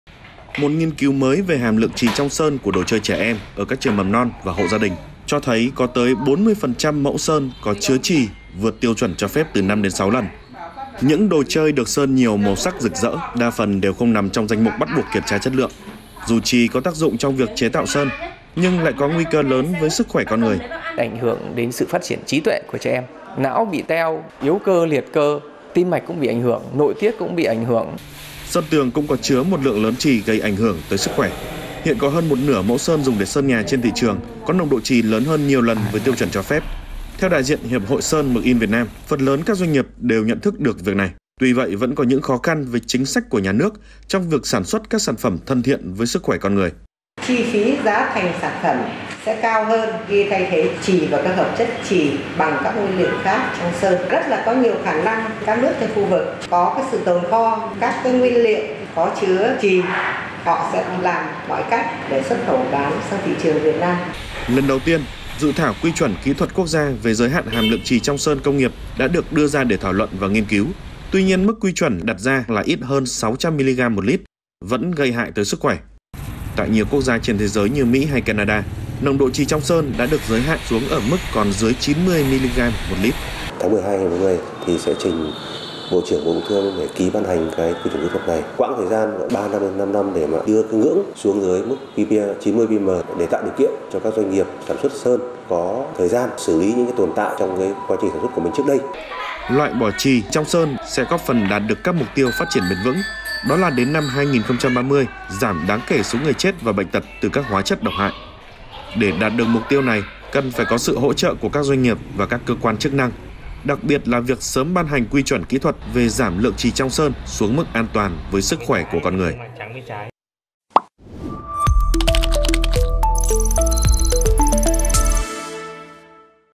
40% mẫu sơn trên đồ chơi của trẻ em chứa lượng chì vượt quá 6 lần cho phép | VTV24
Dù chì có tác dụng trong việc chế tạo sơn nhưng lại ảnh hưởng rất lớn tới sức khỏe con người.